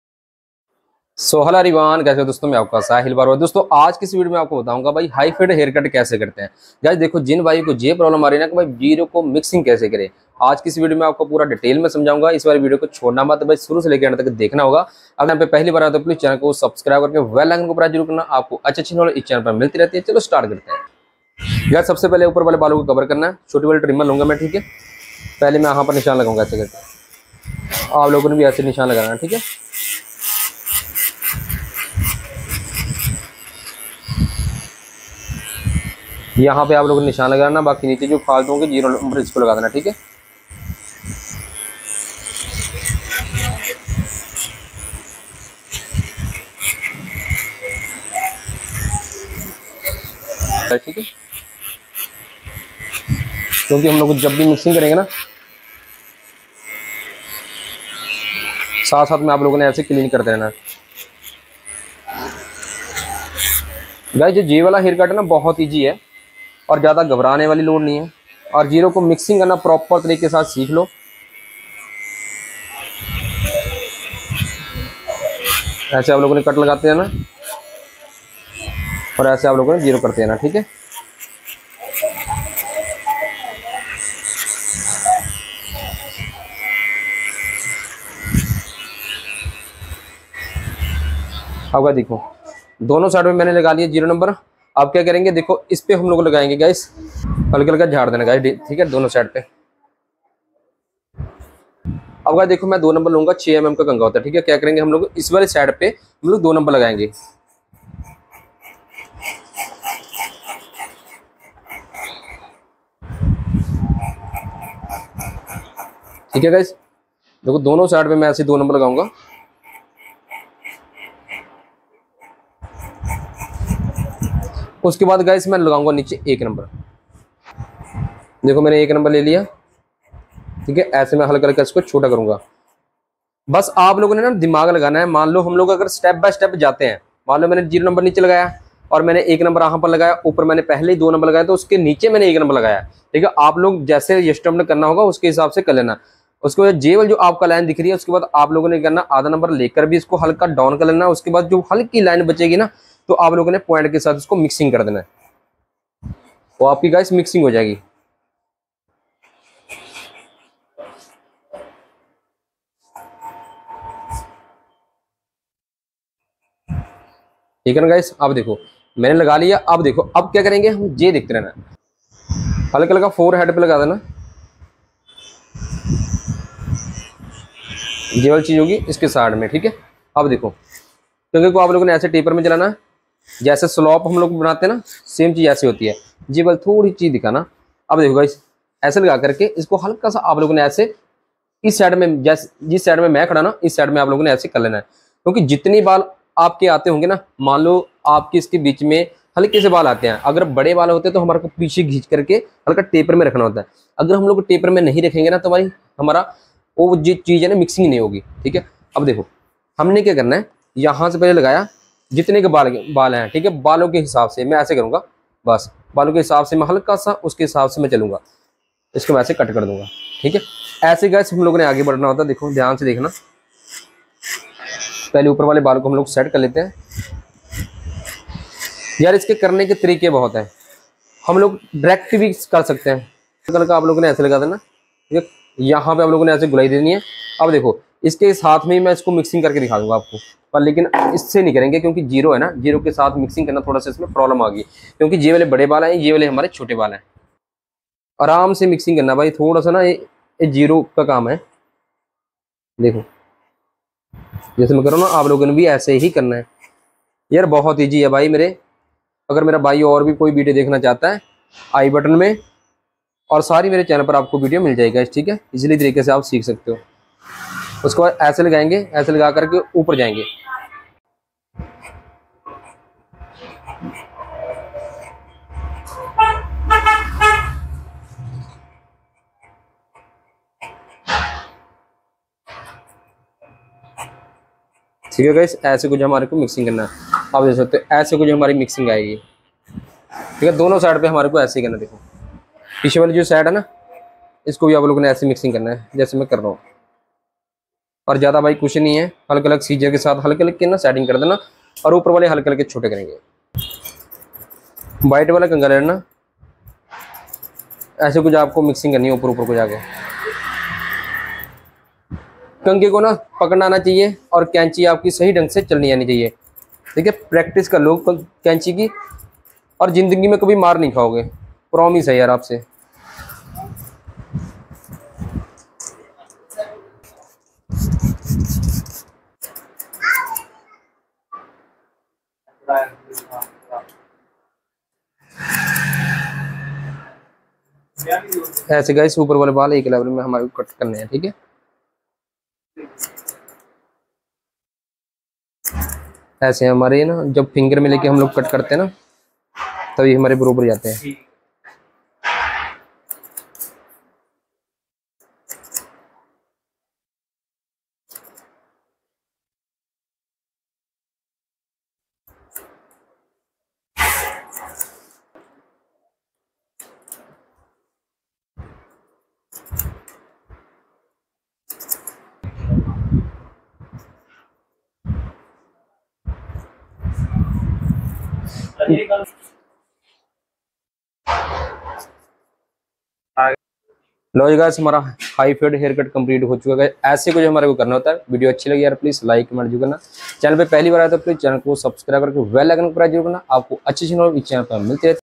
High Fade Haircut Ko ( Mixing Karna Sikhe ) Step by step Tutorial in Hindi | High fade Hairstyle
Apne Bhai Ko Follow Karo Please

Second Channel

high fade haircut tutorial in hindi video
high fade haircut tutorial in hindi for boy